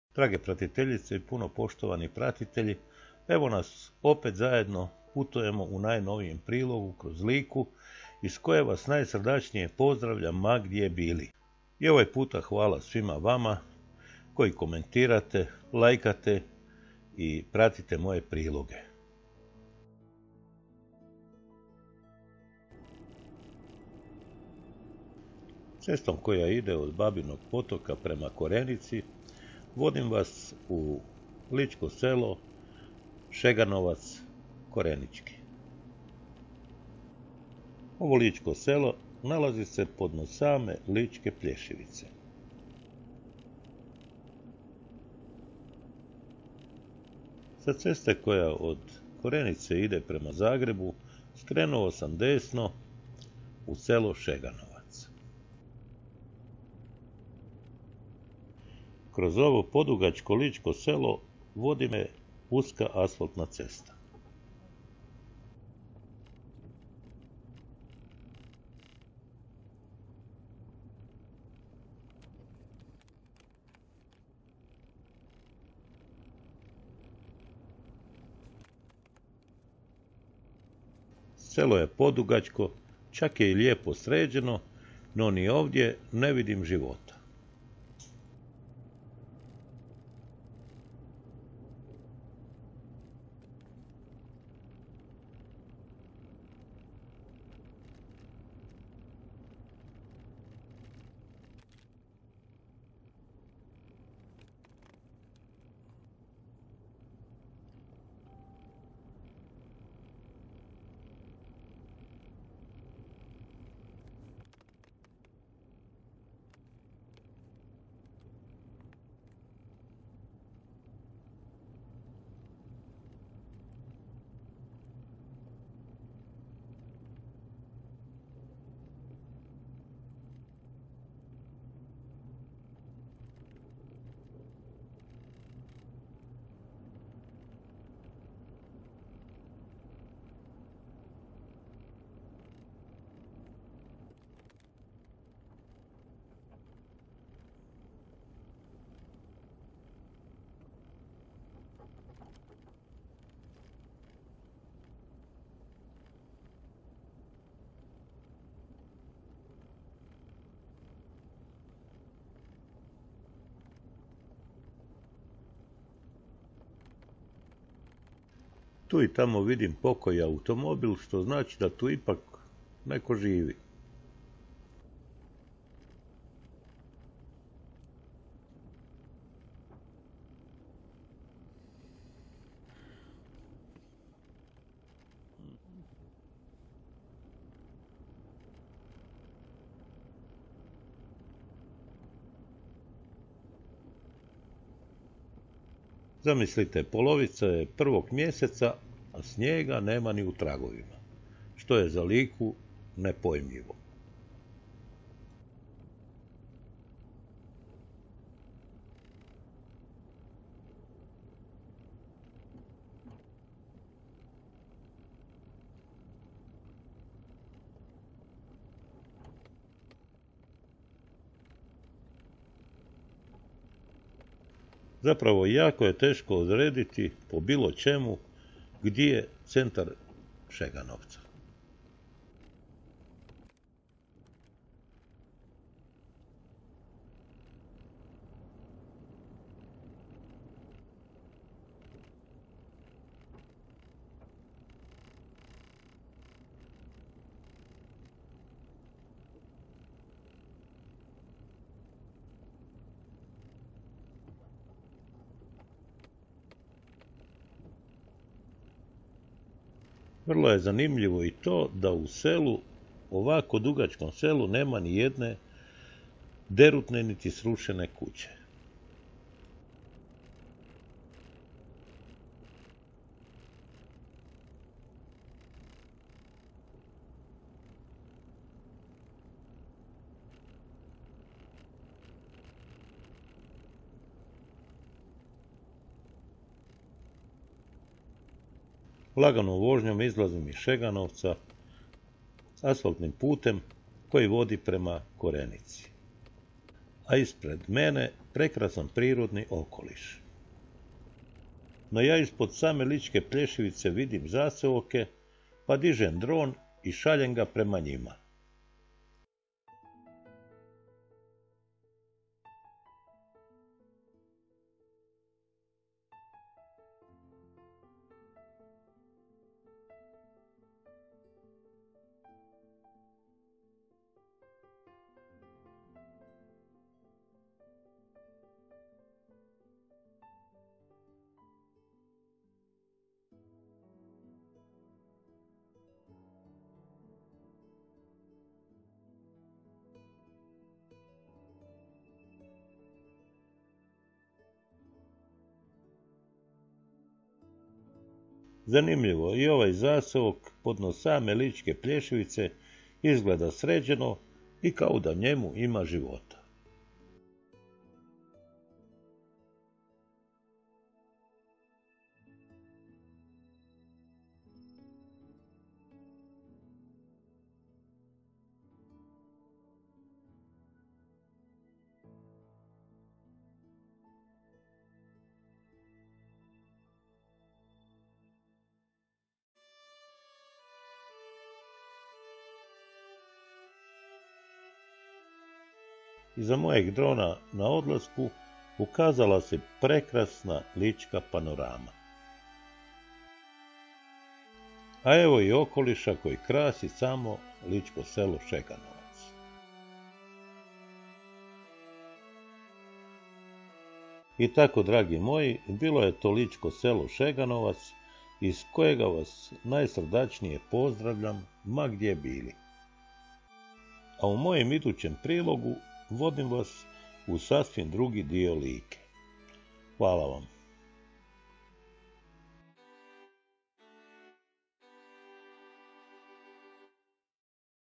LIČKO SELO MIHALJEVAC U ZIMSKOM SNU
Ličko selo Šeganovac ispod same je Gole plješivice a u neposednoj blizini Korenice. Jako lijepo je to Ličko selo sa prekrasnim okolišem a u blizini plitvičkih jezera.